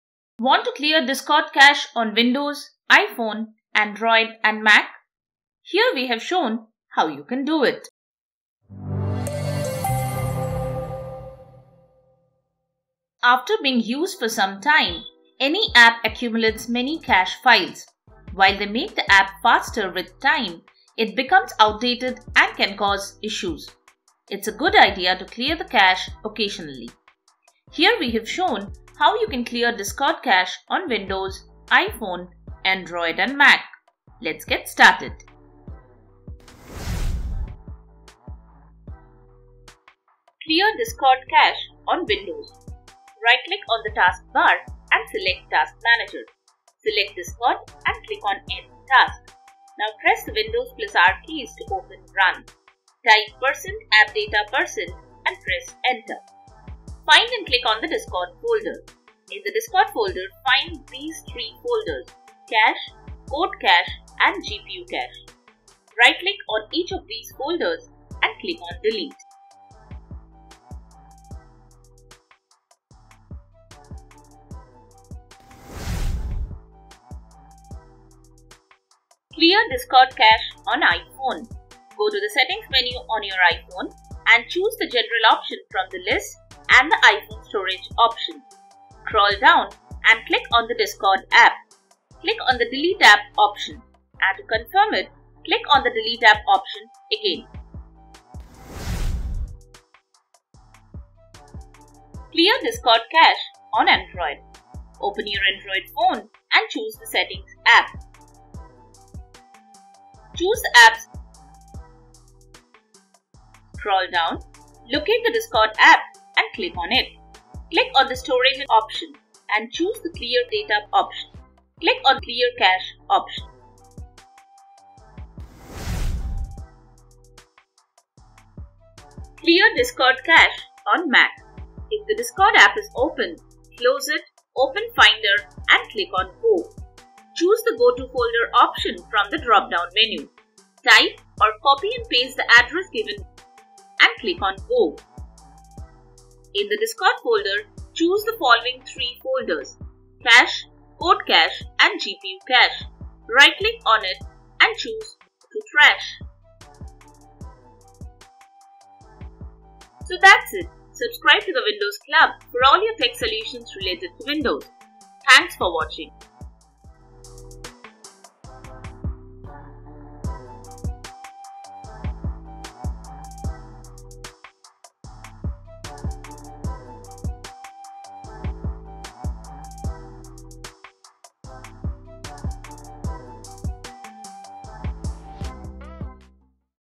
How to clear Discord cache on Windows, iPhone, Android, Mac
After being used for some time, any app accumulates many cache files. While they make the app faster, with time, it becomes outdated and can cause issues. The Discord app is no exception, and since it relies heavily on cached files, it’s a good idea to clear the cache occasionally.

This tutorial will share steps to help you to clear the Discord cache on Windows, iPhone, Android, and Mac.